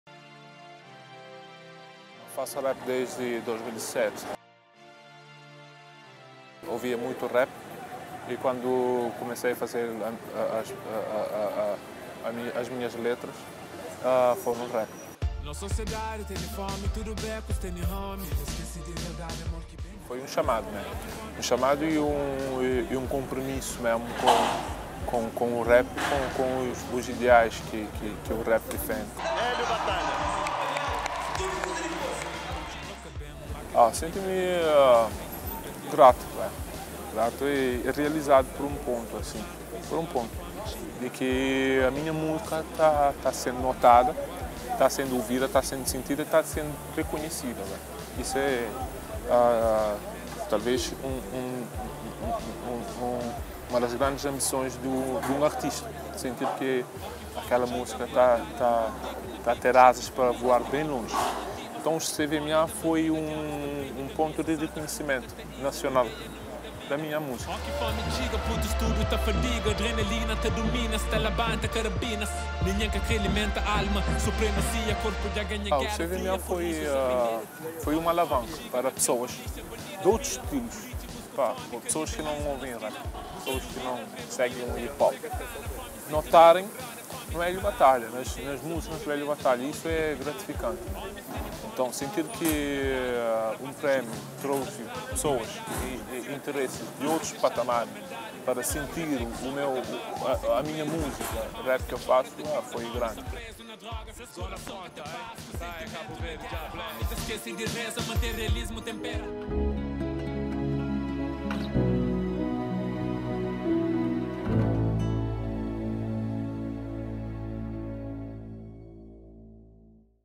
Promo CVMA by Unitel T+ 2017- HÉLIO BATALHA
Os CVMA by Unitel T+ contados na primeira pessoa- HÉLIO BATALHA
Nossa Música, Nossa Identidade
06 de Maio - Praia.

© Produção Executiva: Gilyto Semedo / Zito Sequeira (GMS Entertainment)